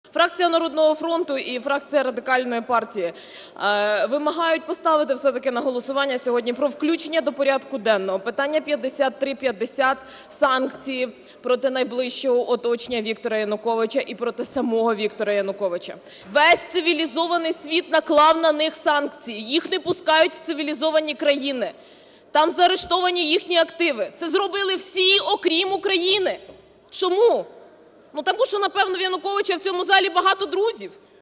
Голосування за санкції показало, що у Януковича в парламенті багато друзів, - Вікторія Сюмар
Верховна Рада має проявити солідарність та розглянути питання введення санкцій проти Віктора Януковича та його найближчого оточення.

Про це заявила заступник голови фракції "Народний фронт", Голова Комітету з питань свободи слова та інформаційної політики Вікторія Сюмар під час виступу з трибуни парламенту.

"Вимагаємо включити до порядку денного питання №5350 щодо санкцій проти найближчого оточення Віктора Януковича і проти нього самого", - наголосила вона.

"Весь цивілізований світ, усі цивілізовані країни заарештували їх активи, це зробили усі, крім України. Чому? Та, напевне, тому, що у Януковича в цьому залі багато друзів", - підкреслила представник "Народного фронту".

За результатами голосування законопроект так і не набрав необхідної кількості голосів.

Приєднуйся!